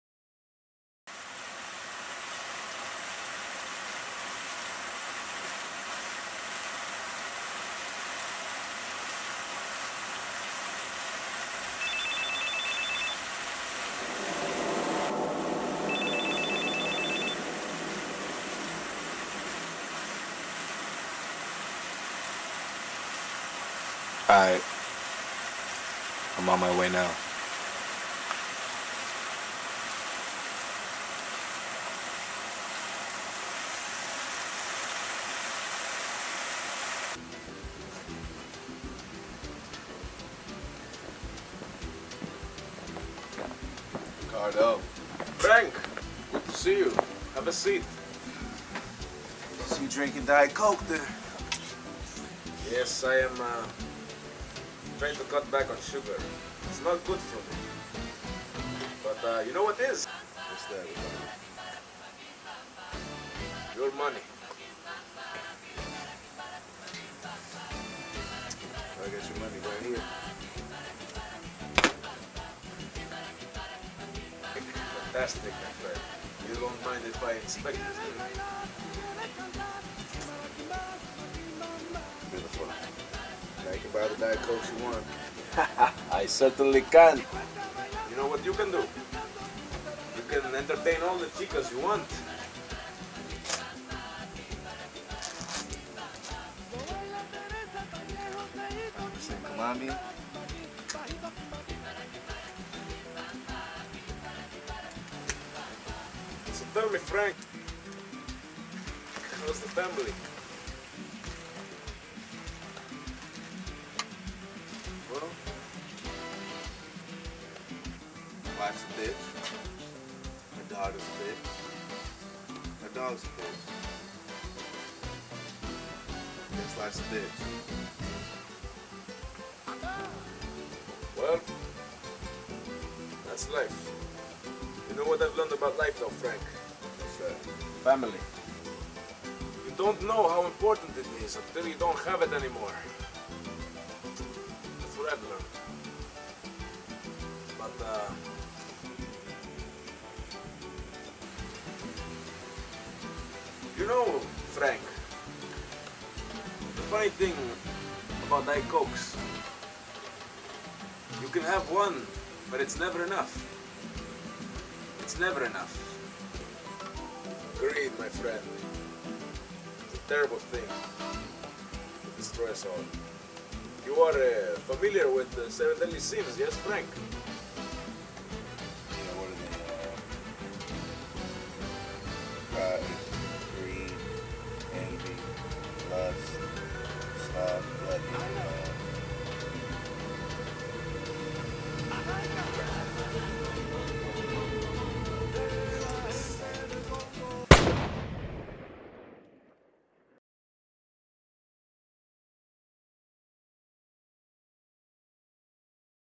Epic J Productions - Jeremy Carter's "Coke"
This is a short movie, written and directed and starred by my brother-from-a-black-mother Jeremy Carter. I just acted in it and did like half of the cinematography. He took care of all the editing and the directing and half the shots as well.